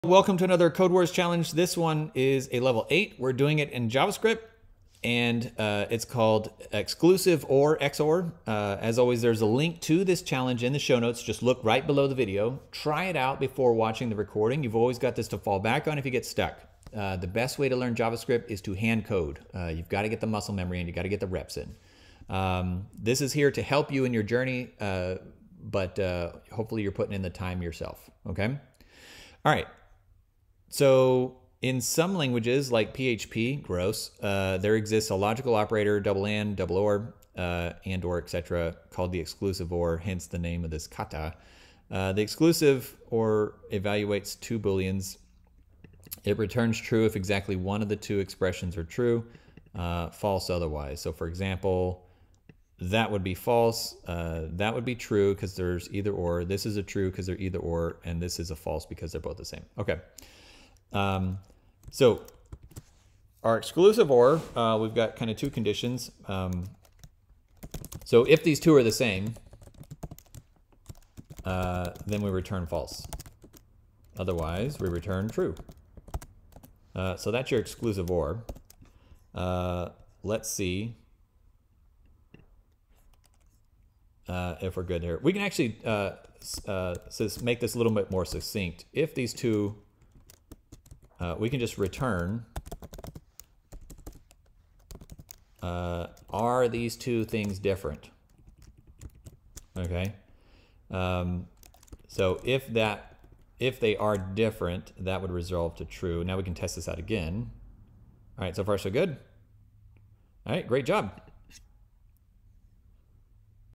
Codewars - Javascript - Exclusive "or" (xor) Logical Operator
Code along with me as we solve 'Exclusive "or" (xor) Logical Operator', a Level 8 kyu javascript codewars challenge. Here's a link to the challenge